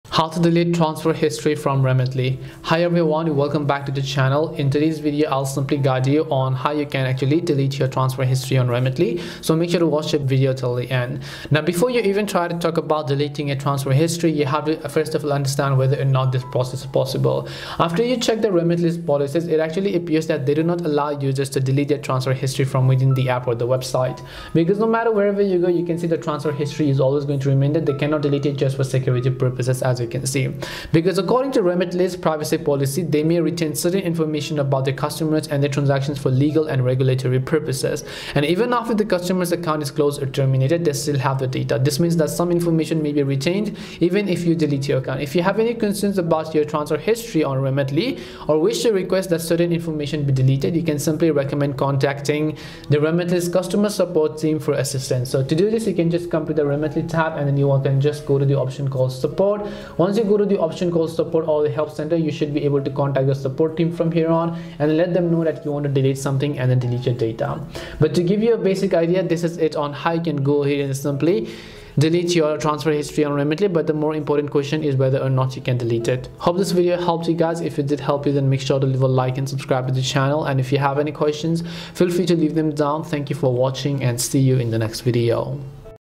How to Delete Transfer History from Remitly !
In this tutorial video, I am simply going to show you how you can delete your transfer history on Remitly.

~Chapters:
0:00 Introduction
0:11 Delete Transfer History
1:17 Conclusion